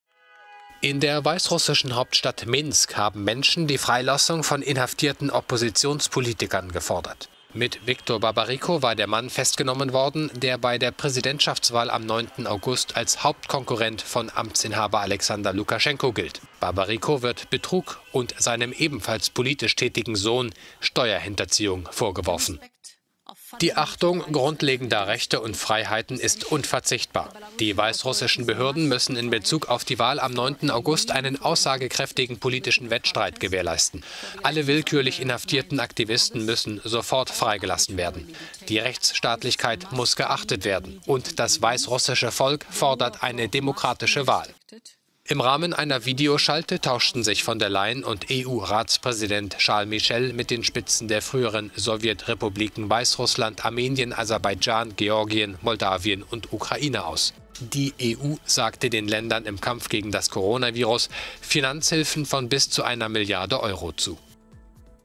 Weißrussland: Protest gegen Verhaftung von Oppositionspolitikern
Unterdessen tauschten sich die EU-Spitzen mit den Vertretern des Landes und weiterer früherer Sowjetrepubliken in einer Videoschalte aus.